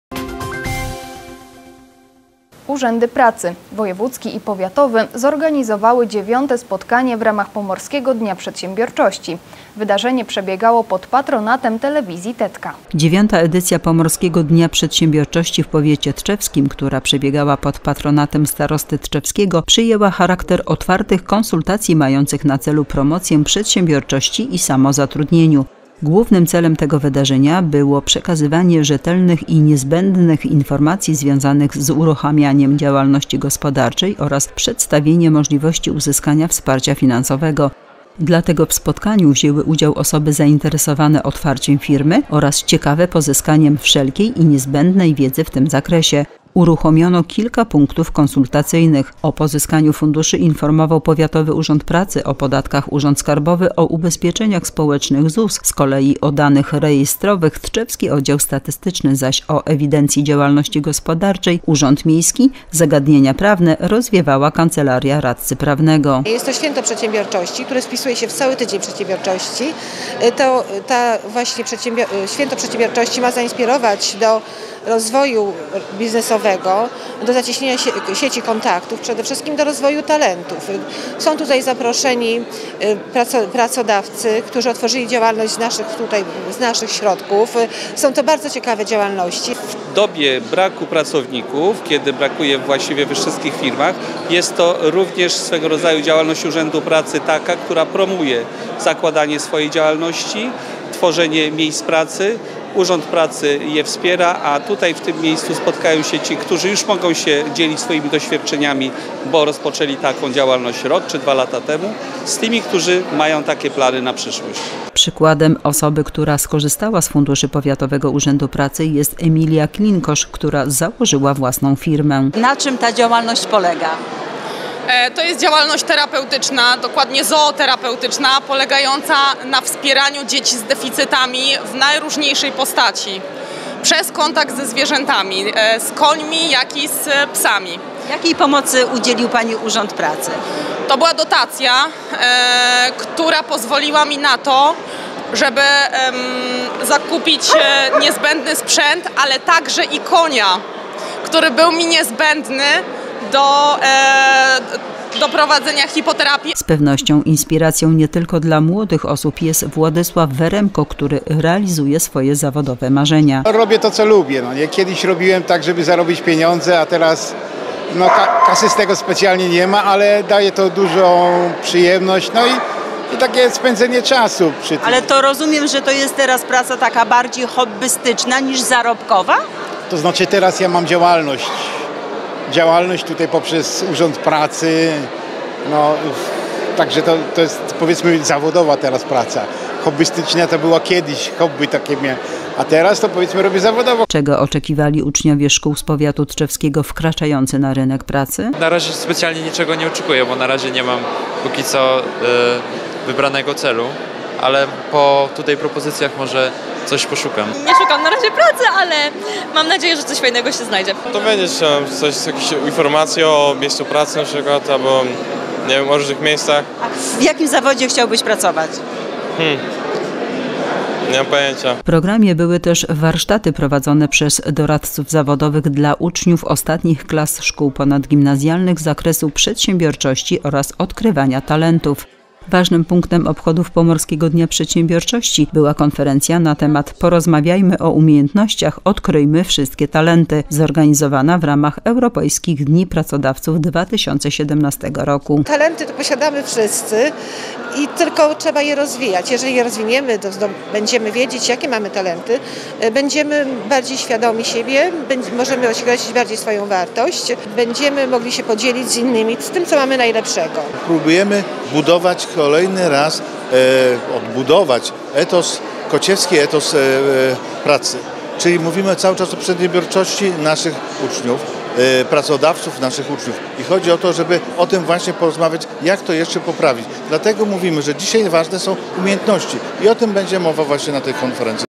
Od pomysłu do biznesu - Tv Tetka Tczew HD
Urzędy pracy – Wojewódzki i Powiatowy – zorganizowały dziewiąte spotkanie w ramach Pomorskiego Dnia Przedsiębiorczości. Wydarzenie przebiegało pod patronatem Telewizji TeTka.